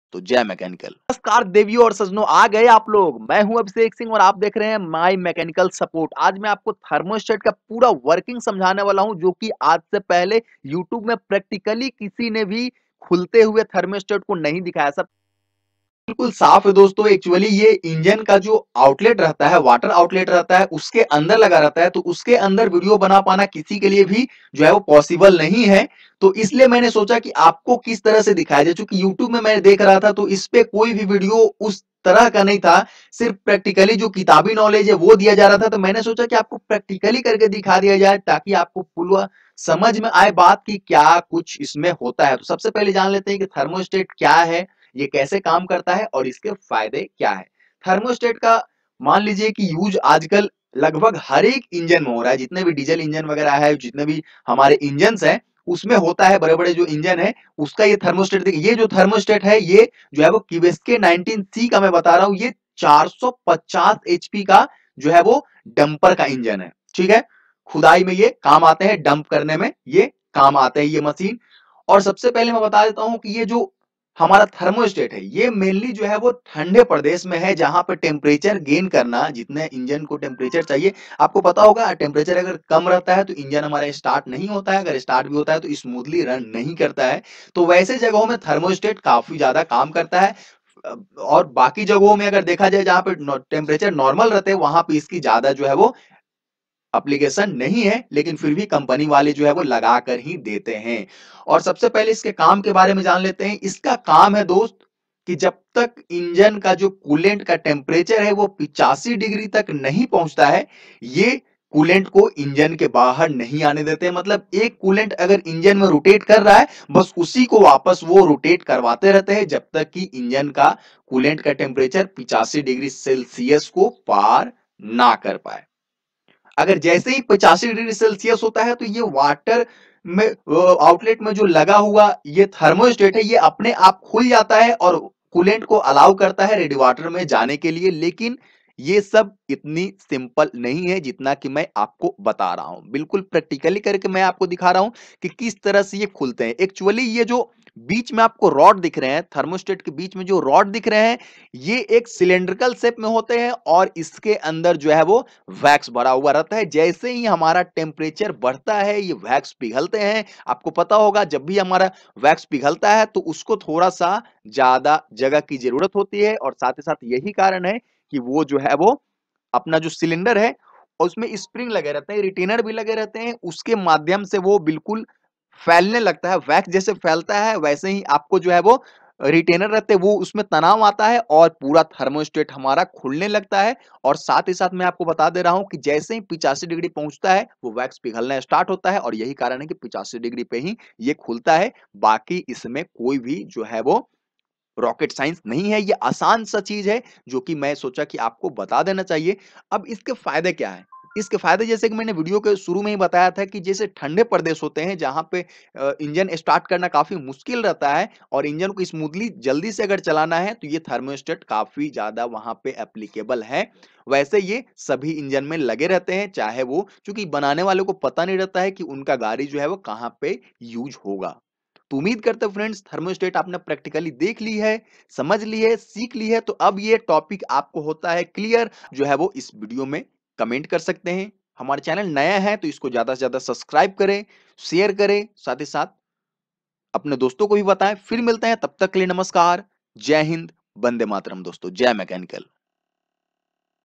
Thermostat | Working Of Thermostat | Advantages Of Thermostat | In Hindi
in this video know about working of thermostat and how it opens and also know the working principal of thermostat in diesel engine and other heavy vehicle in the coolent it is use to open and close outlet valve of coolent